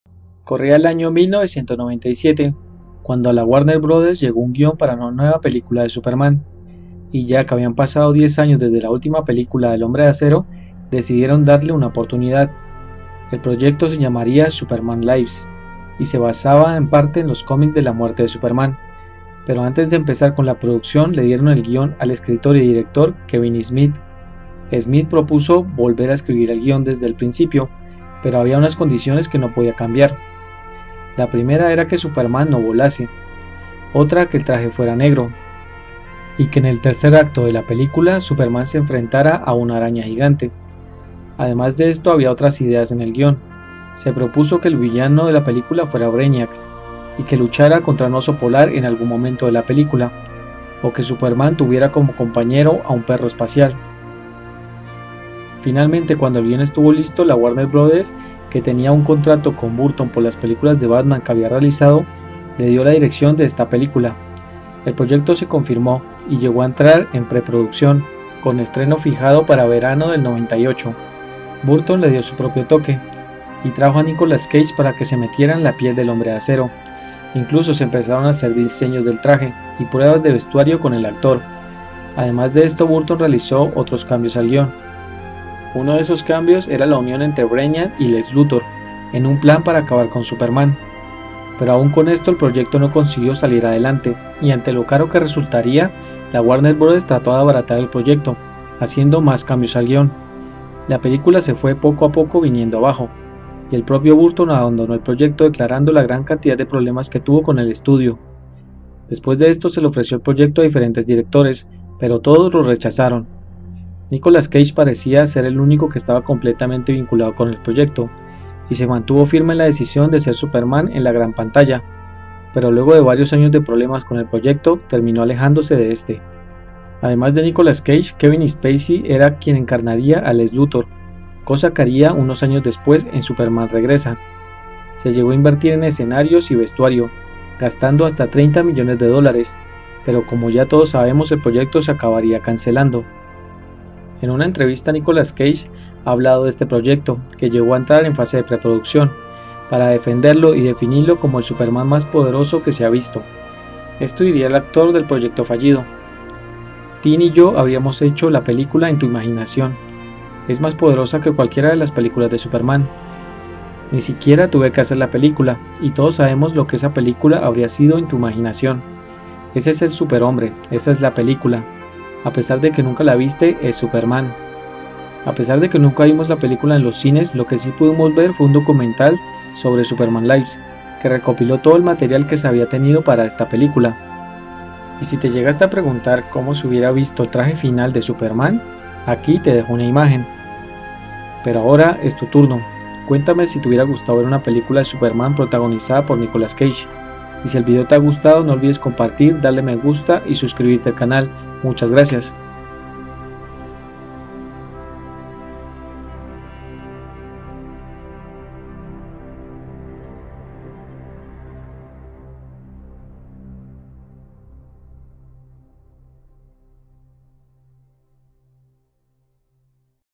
“SUPERMAN LIVES” la película del HOMBRE DE ACERO que nunca vimos
Corría el año de 1997 cuando ala Warner bros llego un guion para una nueva película de Superman y ya que habían pasado 10 años desde la última película del hombre de acero decidieron darle una oportunidad,  el proyecto se llamaría “Superman Lives” …

MUCHAS GRACIAS...